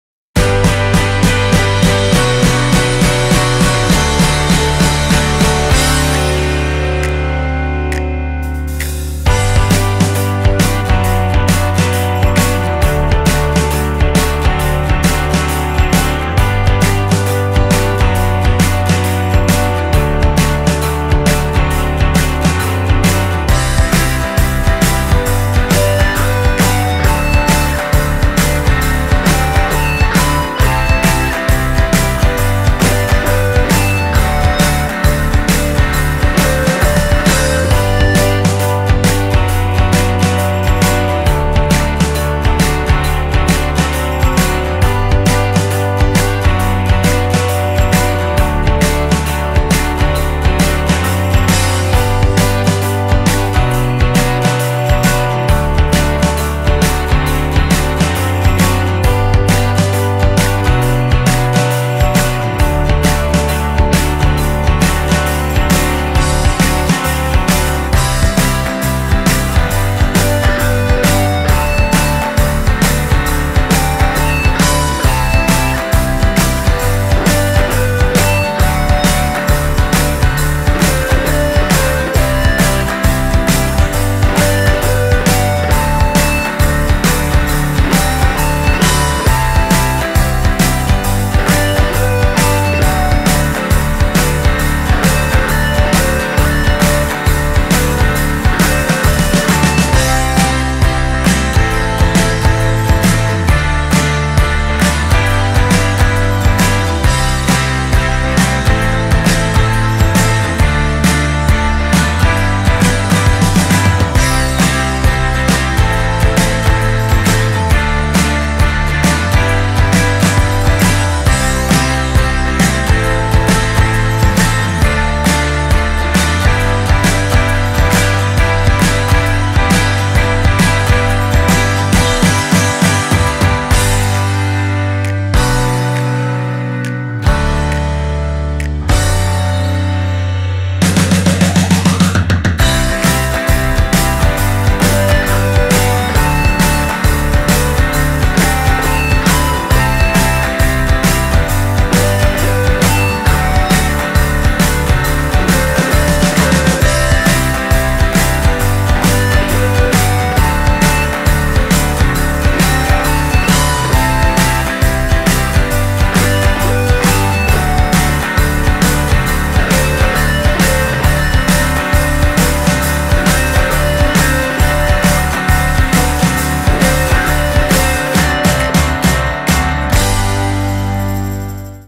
Harte Kante und steife Brise
Das erste Training wart hart, kalt und sonnig. Bei 1° und eisigem Nord-Westwind bat Stefan Krämer zum ersten Training. Es ging mitunter hart, dennoch immer fair zur Sache. Das Trio - Ledgerwood, Hübener und Garbuschewski trainierte zunächst noch separat mit Fitnesscoach Christian Behring. Der Rest trennte sich zum Ende der Einheit im Trainingsspiel leistungsgerecht mit 2:2. Seht selbst...